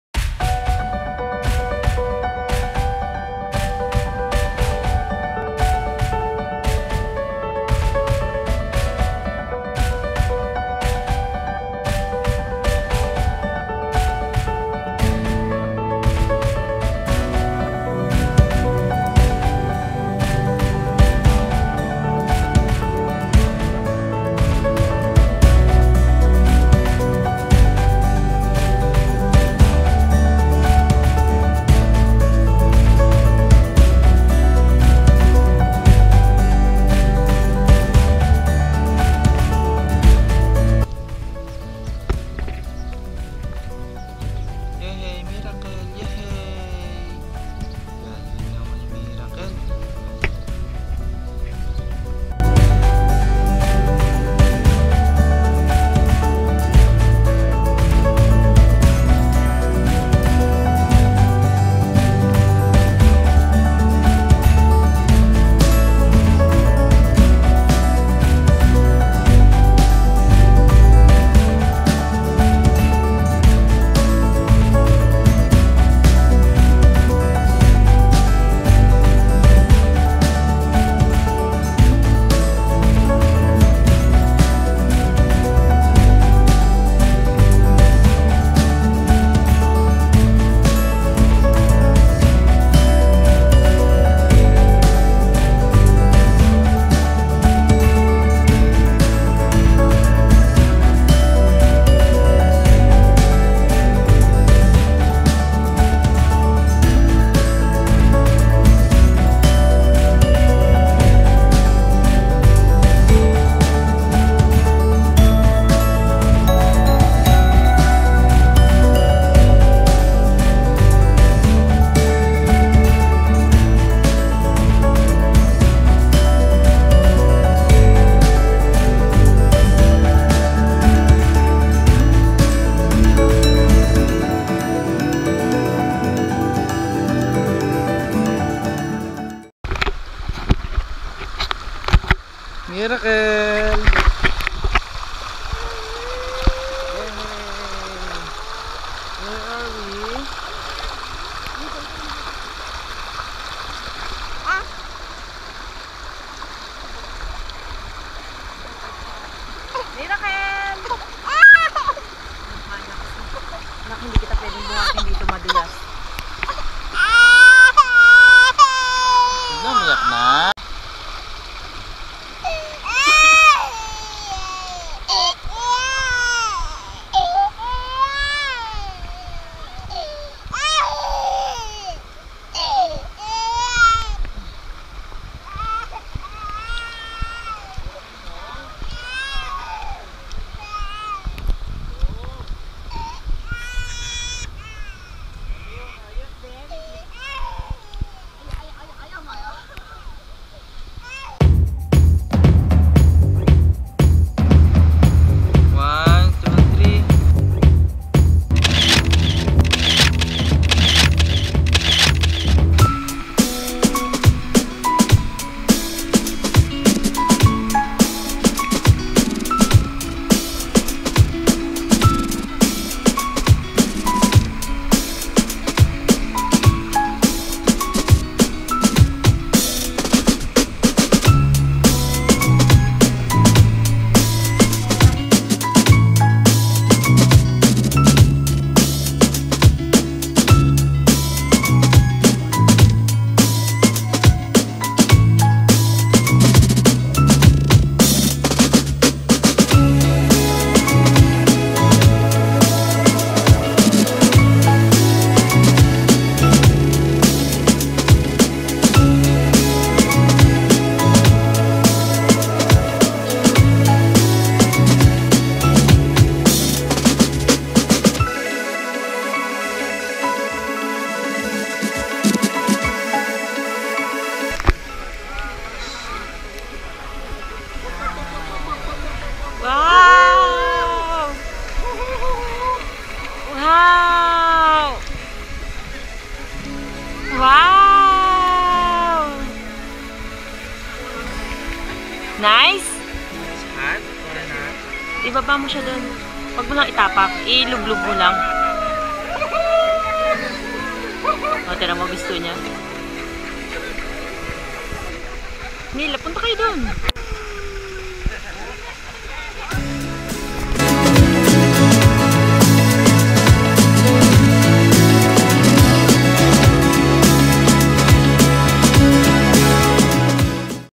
Exploring Turkey with My Family | Day 5: Pamukkale-Hierapolis, Turkey | MIRACLEventures
Hi, my name is Reign Miracle Reyes and i like to travel and eat a lot!
This is the last part of my Turkey Adventure!
Did you know that "Pamukkale" in English means "COTTON CASTLE"? 🤔

Day 5: Pamukkale-Hierapolis, Turkey
-Great Roman Theatre
-Cleopatra Antique Thermal Pool
-Cotton Castles